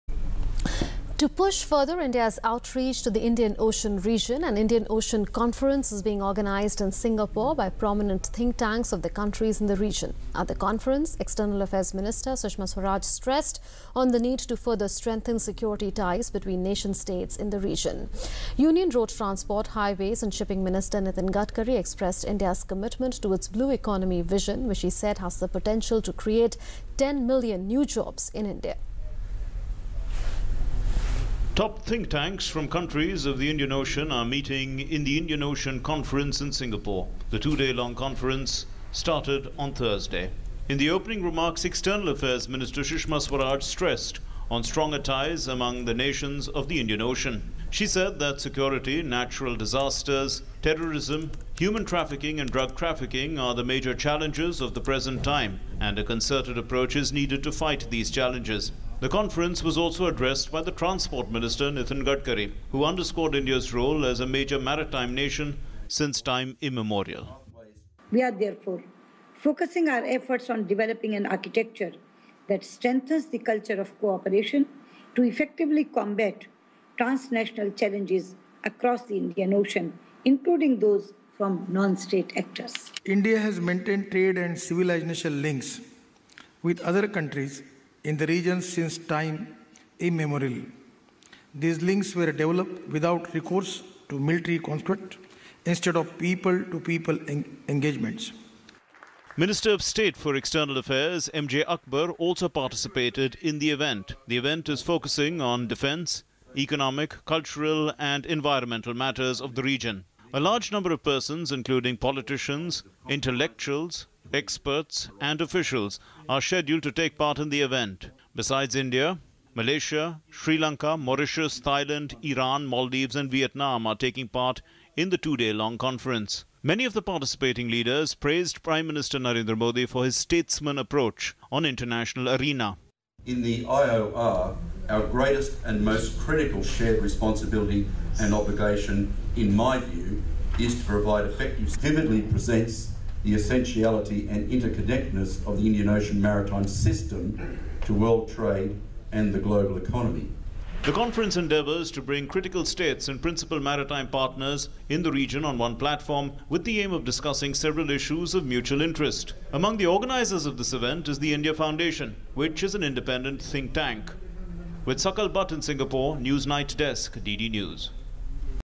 Indian Ocean Conference is being organised in Singapore
To push further India's outreach to the Indian Ocean region, an Indian Ocean Conference is being organised in Singapore by prominent think tanks of the countries in the region. At the conference, External Affairs Minister Sushma Swaraj stressed on the need to further strengthen security ties between nation states in the region